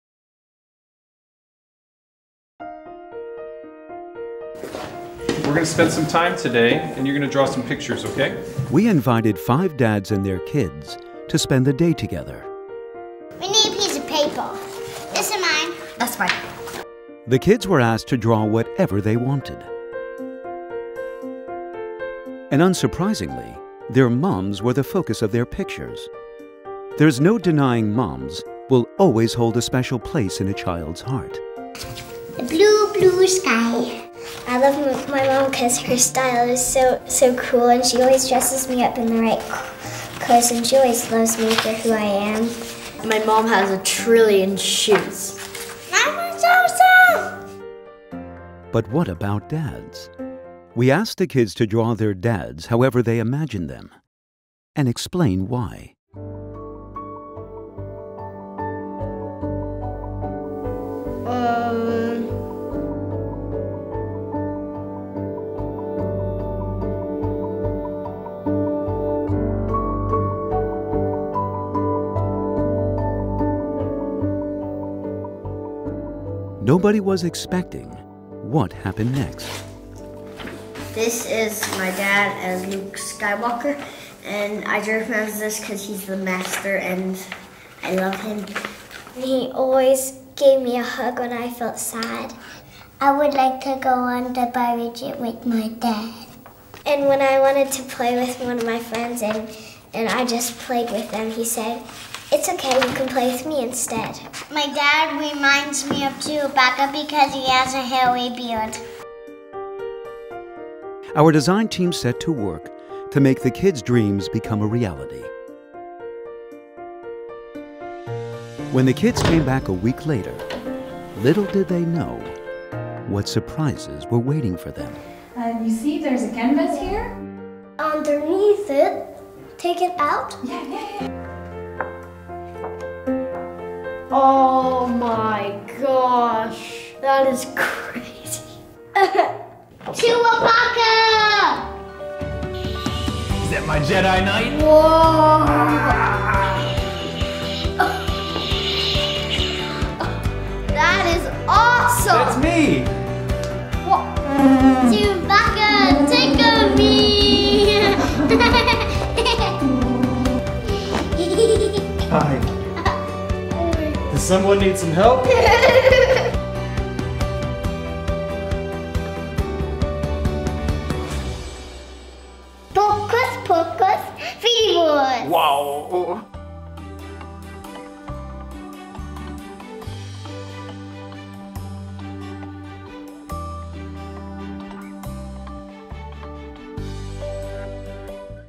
I papà visti con gli occhi dei loro bambini Stylight video
Come sono i papà visti con gli occhi dei loro bambini? Qui a Stylight, abbiamo  chiesto a un gruppo di bambini dai 4 agli 8 anni di disegnare i loro papà così come se li immaginano. Preparatevi per l'effetto sorpresa, tanti sorrisi e qualche lacrimuccia!

Scopri cosa provano i bambini per i loro papà! ► Guarda il video e scopri tutti i dettagli qui

► I bambini disegnano i loro papà: preparatevi per sorrisi e lacrimucce!
• I bambini disegnano i loro papà come supereroi, personaggi fantastici e molto altro
• Il risultato è adorabile e divertente
• I disegni vengono digitalizzati dai graphic designer del team di Stylight
• I papà si travestono così come i bimbi se li immaginano
• Sorpresa!